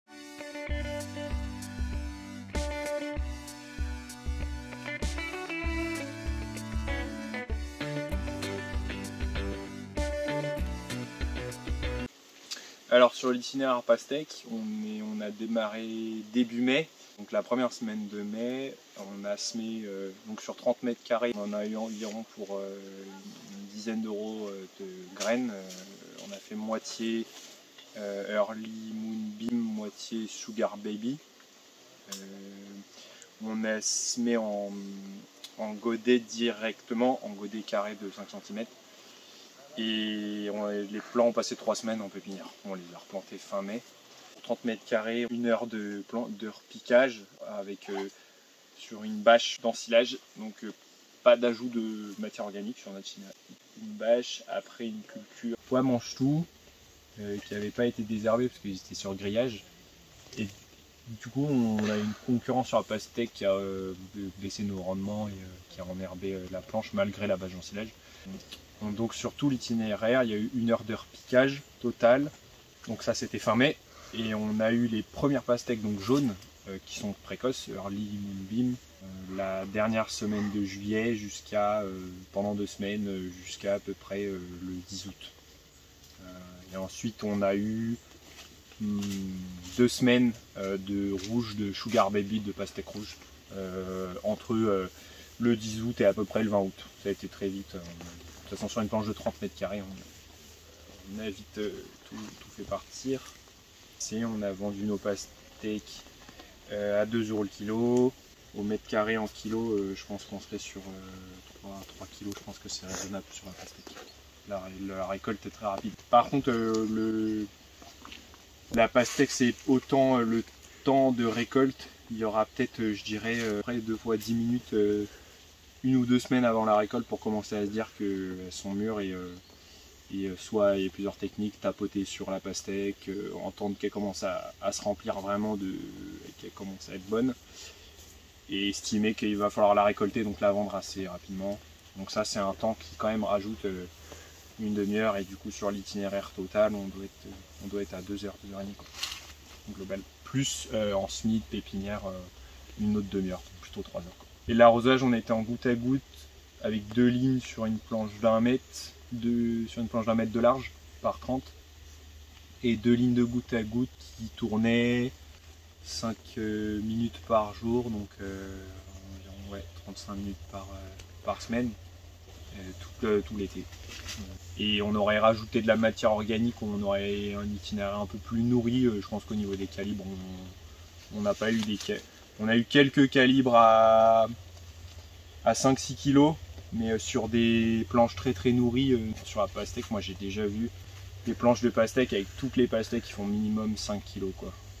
Fiche ITK - Pastèque sur bâche d'ensilage - Ferme de l'Alliance
George, en espace test sur la Ferme de l'Alliance, nous présente son itinéraire technique pour la pastèque sous serre.

Vous pouvez retrouver la fiche technique avec les marges brutes sur Triple Performance